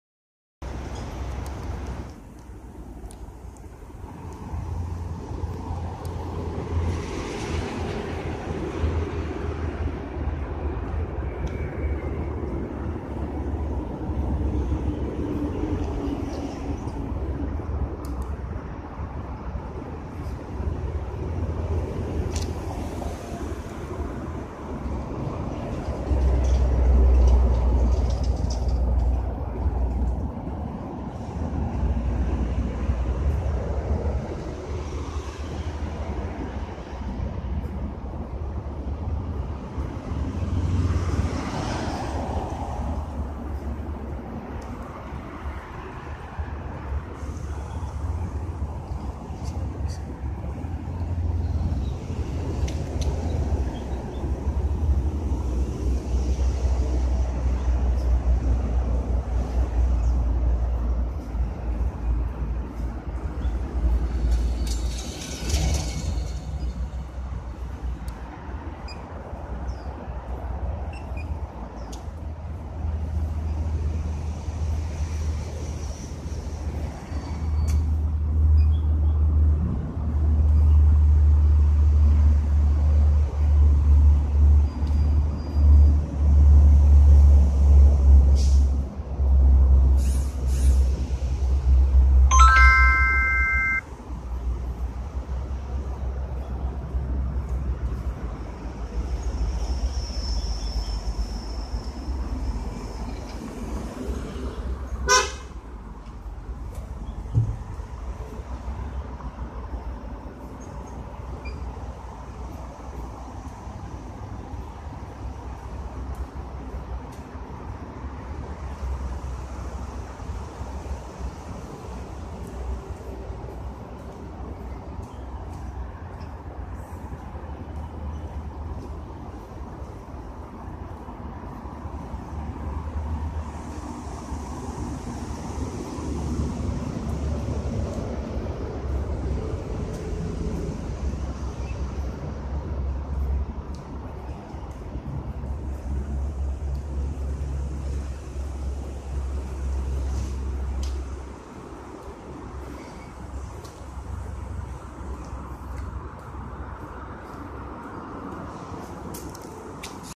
ofo bike logistic coordination
only by one driver, one truck, one boy